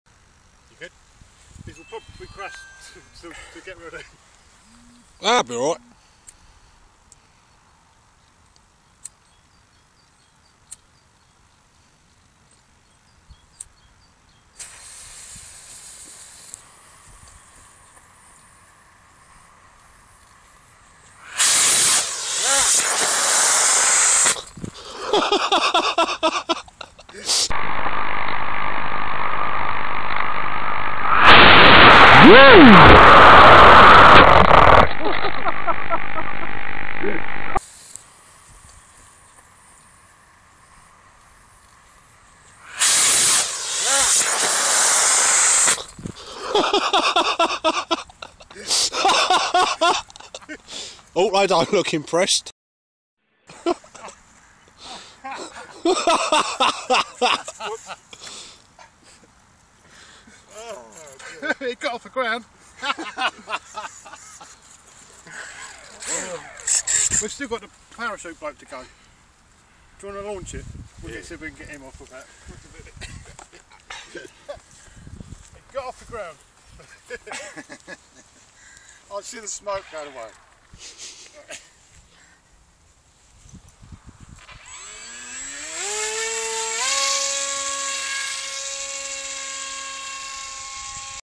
AXN Rocket Powered Vertical Take Off
Can you launch an  axn with a rocket , well we got it about 4 to 5 foot before it all went pete tong . Still she lives on